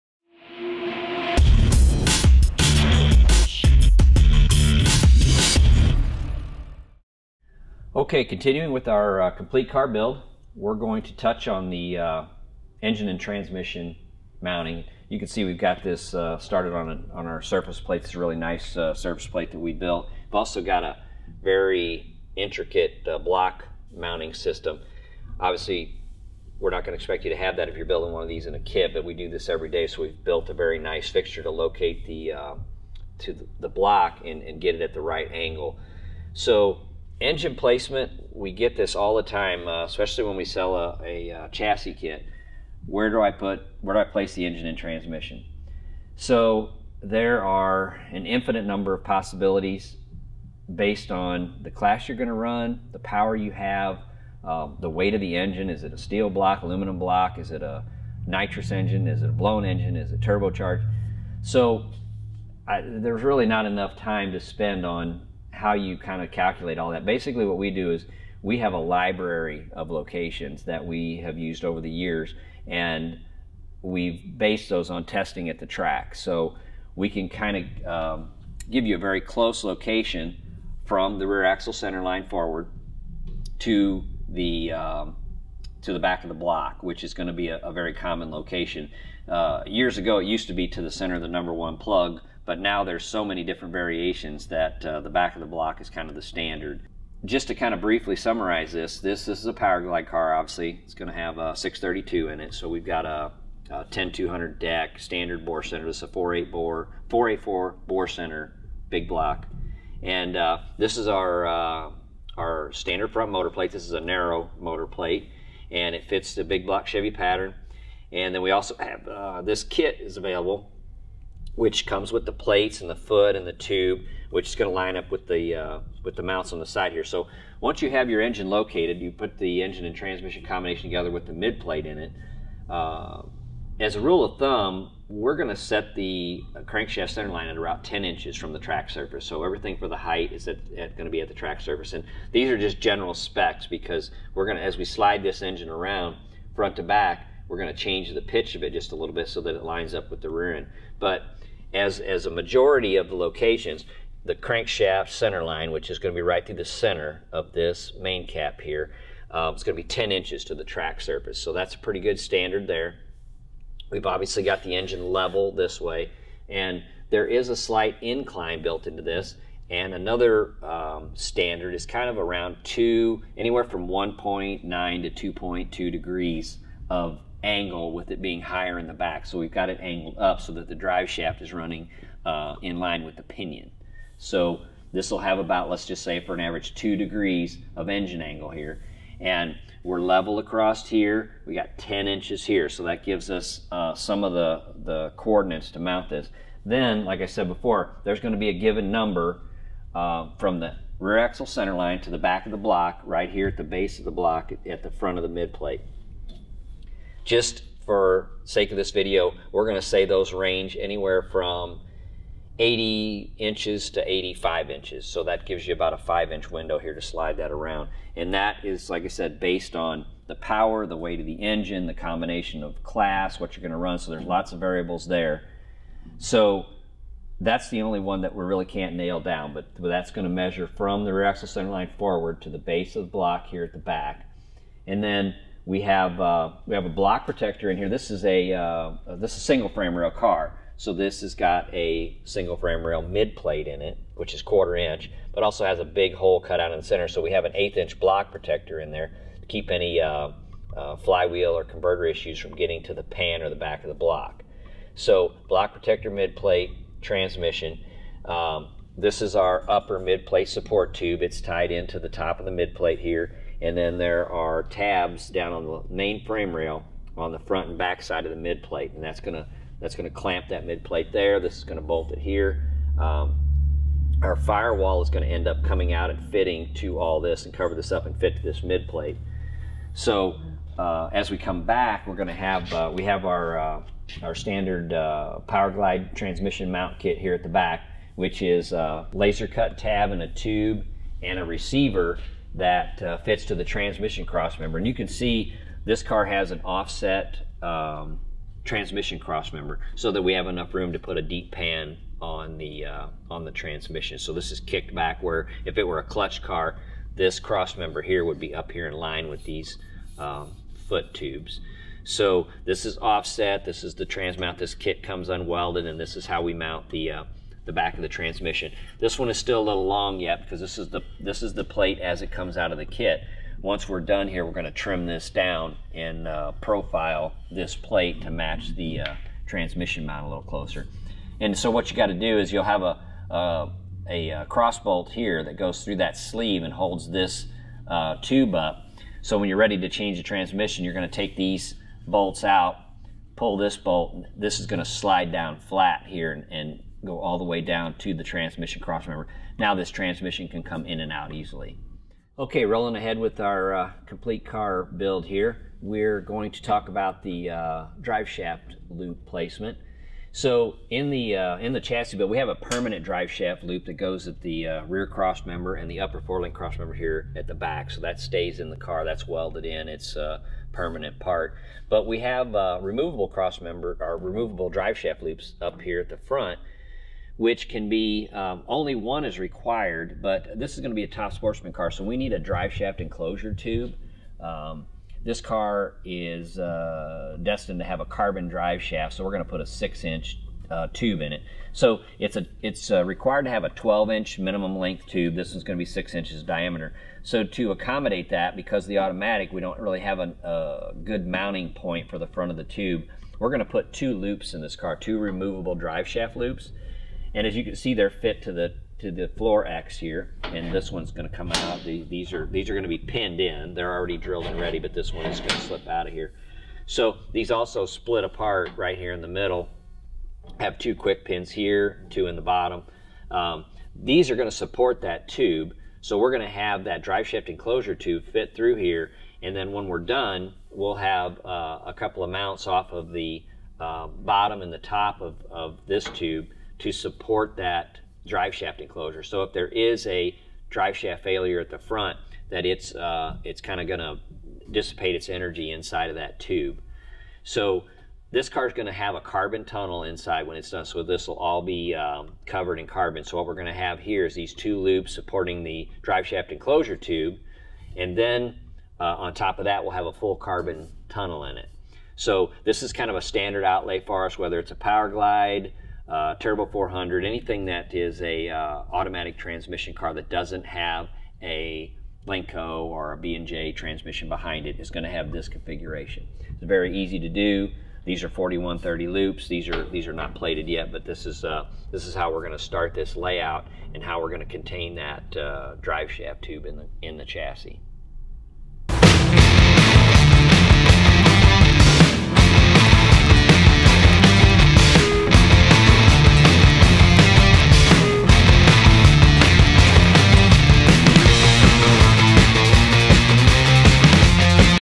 Door Slammer Build: 02 - Engine & Transmission Mounting
This week in our Door Slammer Build Series, Tim demonstrates how to locate and mount the engine and transmission.